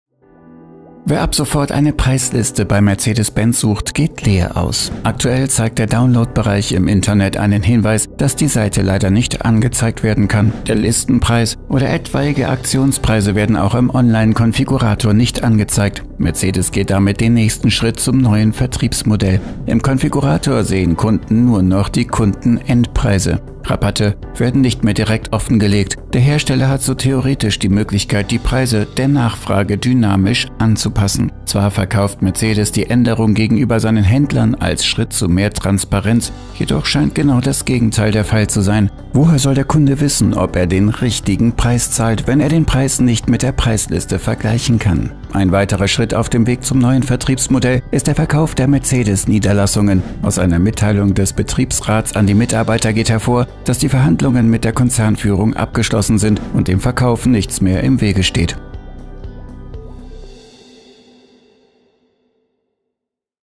Völlig intransparent: Mercedes versteckt seine Preislisten
Mercedes zeigt seinen Kunden ab sofort keine Preislisten mehr. Begründet wird der Schritt mit mehr Transparenz – offenbar ist das Gegenteil der Fall.

Dieses Video entstand durch eine Kooperation mit BitProjects.

EFAHRER.com ist eine Plattform, die markenunabhängig zu Themen rund um Elektromobilität informiert.